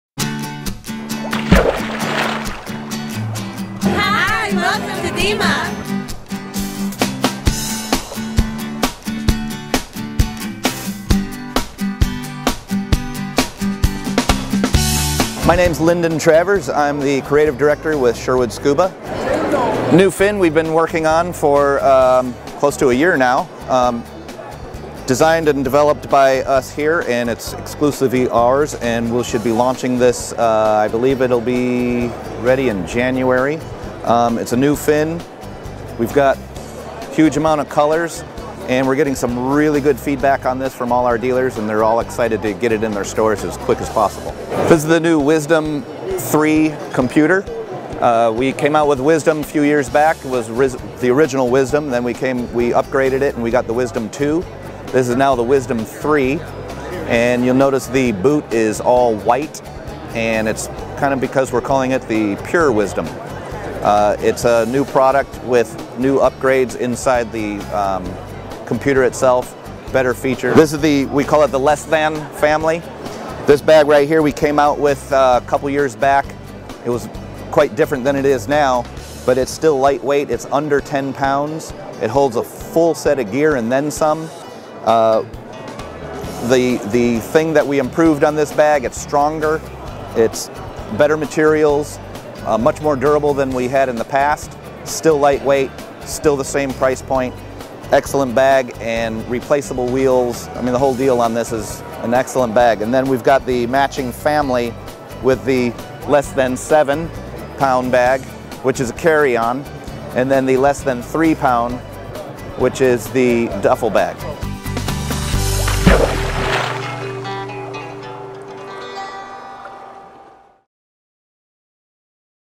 DEMA Show 2011 - What You're Missing (Sherwood Scuba)
DEMA Show 2011 is taking place November 2-5 in Orlando, Florida!  Check out the cool things you're missing and plan to visit us next year, November 14-17, 2012 in Las Vegas, NV!